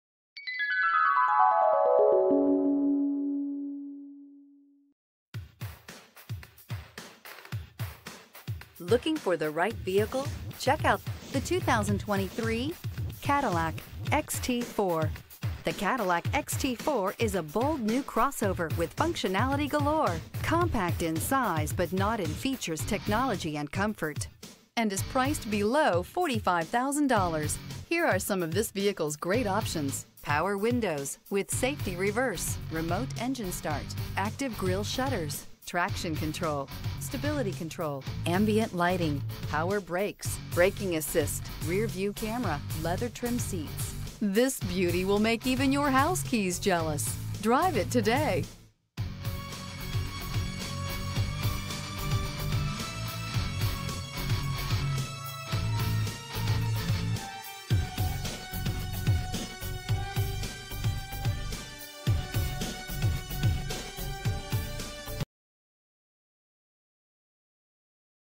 2023 Cadillac XT4 Mooresville NC CA2358
2023 Cadillac XT4 Premium Luxury

You'll love this 2023 Cadillac XT4. This is a vehicle you'll want to take home. Call and get in touch with Randy Marion Chevrolet Buick Cadillac directly, and be the first to open the vehicle door today!